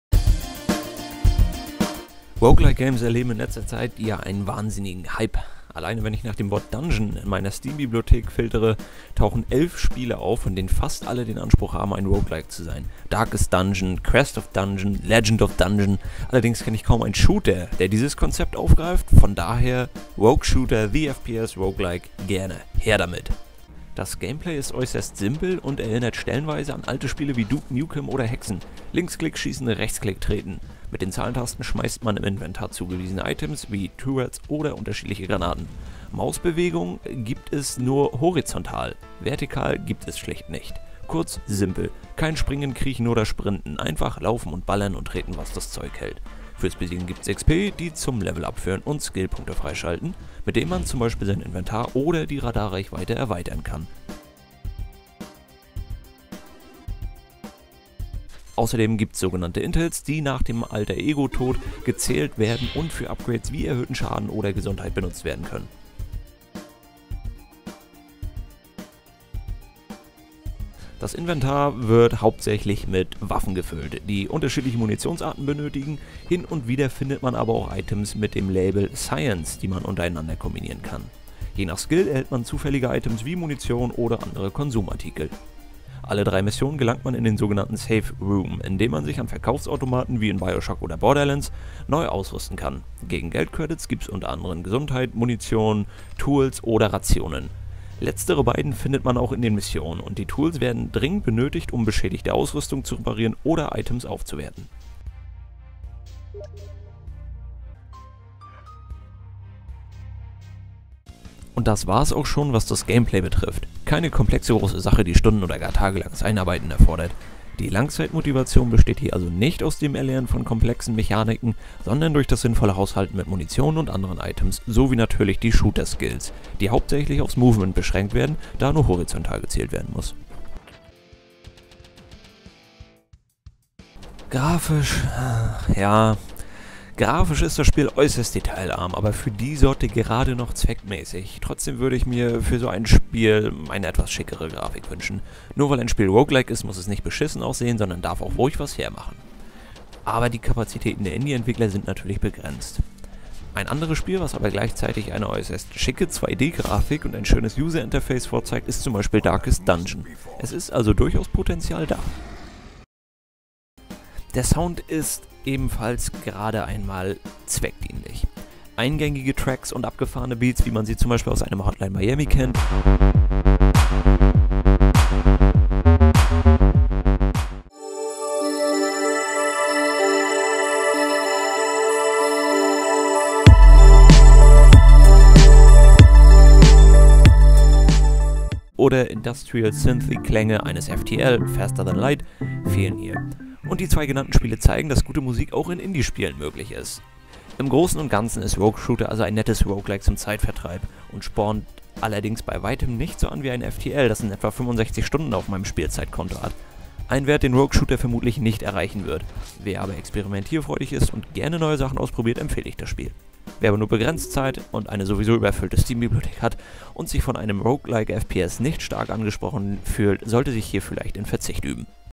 Rogue Shooter: The FPS Rogue Like - Indie Review (Deutsch)
Nach Primordia ein weiteres Indie-Game auf meiner Review-Liste.

Defintiv mal etwas anderes, was aber leider nicht über offensichtliche Schwächen hinwegsehen lässt.